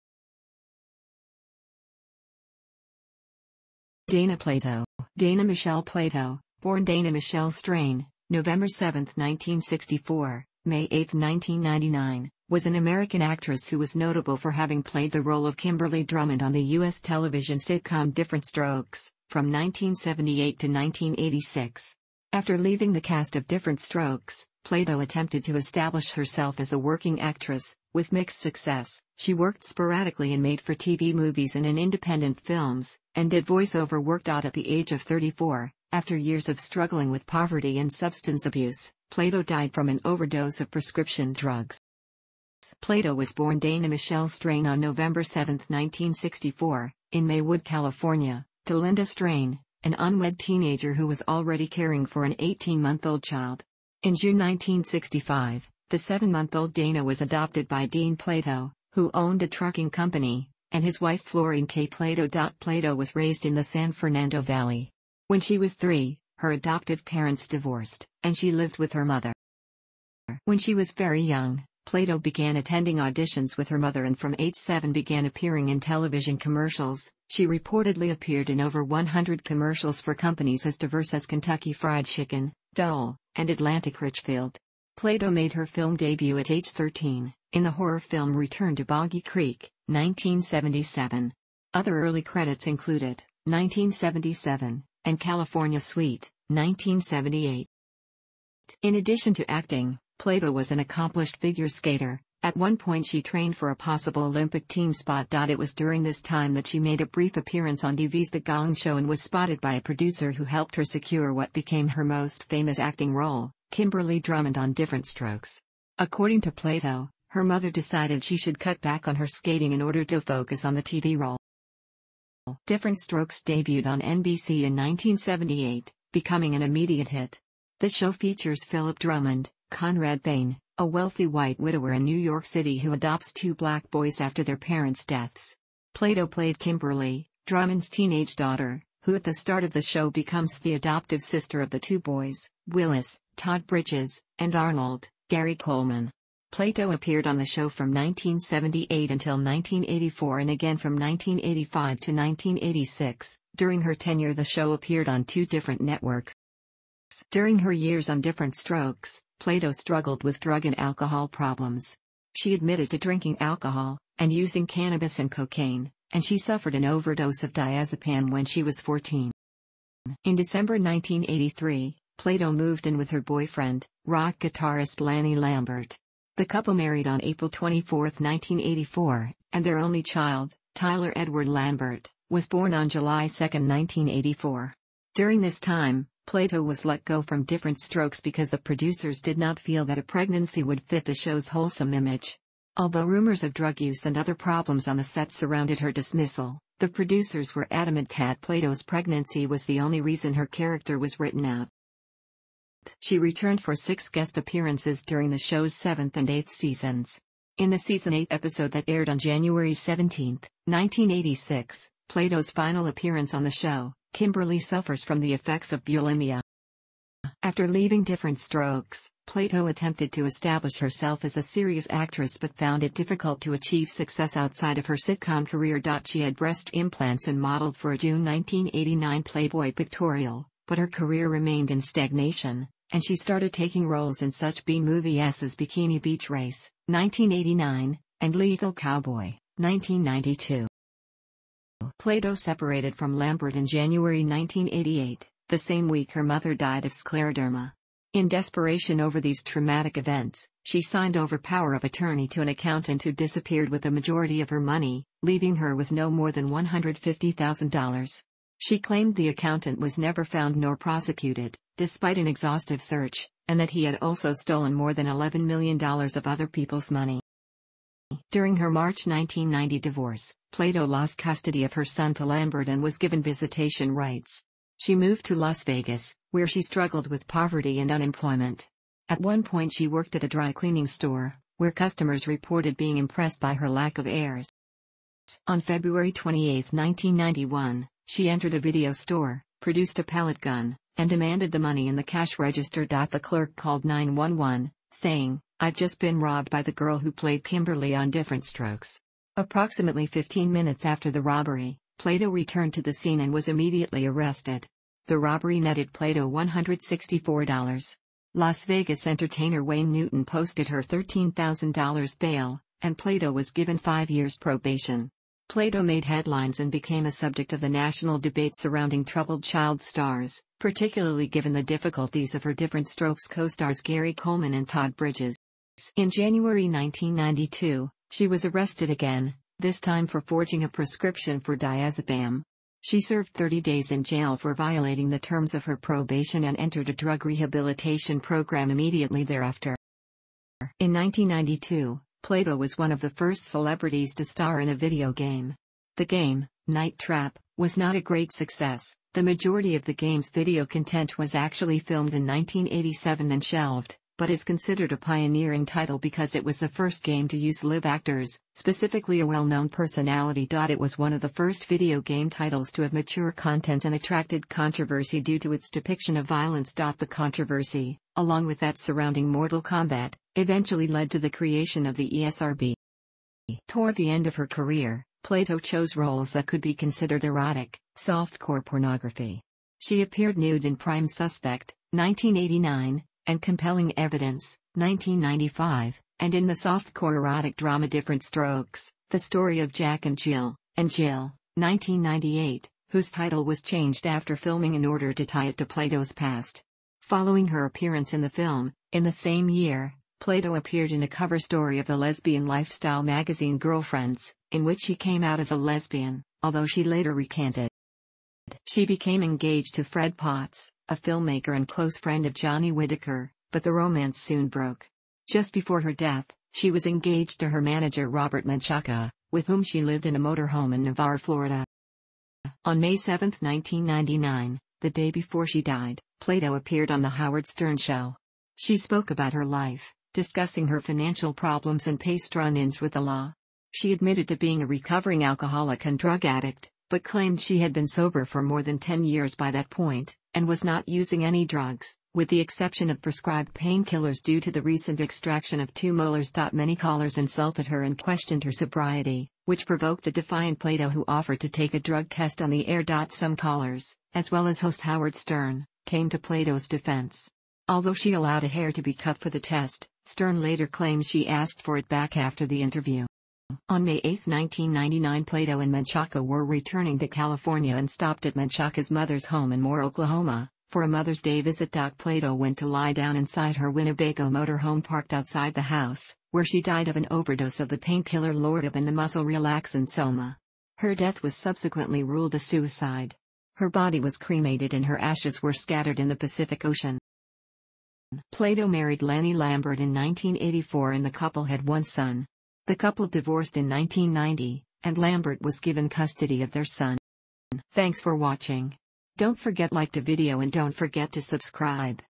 Dana Plato
Dana Michelle Plato (born Dana Michelle Strain; November 7, 1964 – May 8, 1999) was an American actress who was notable for having played the role of Kimberly Drummond on the U.S. television sitcom "Diff'rent Strokes", from 1978 to 1986. After leaving the cast of "Diff'rent Strokes", Plato attempted to establish herself as a working actress, with mixed success: she worked sporadically in made-for-TV movies and in independent films, and did voice-over work. At the age of 34, after years of struggling with poverty and substance abuse, Plato died from an overdose of prescription drugs.

Plato was born Dana Michelle Strain on November 7, 1964, in Maywood, California, to Linda Strain, an unwed teenager who was already caring for an 18-month-old child. In June 1965, the seven-month-old Dana was adopted by Dean Plato, who owned a trucking company, and his wife Florine "Kay" Plato. Plato was raised in the San Fernando Valley. When she was three, her adoptive parents divorced, and she lived with her mother.

When she was very young, Plato began attending auditions with her mother and from age seven began appearing in television commercials; she reportedly appeared in over 100 commercials for companies as diverse as Kentucky Fried Chicken, Dole, and Atlantic Richfield. Plato made her film debut at age 13, in the horror film "Return to Boggy Creek" (1977). Other early credits included "" (1977) and "California Suite" (1978).

In addition to acting, Plato was an accomplished figure skater; at one point she trained for a possible Olympic team spot. It was during this time that she made a brief appearance ...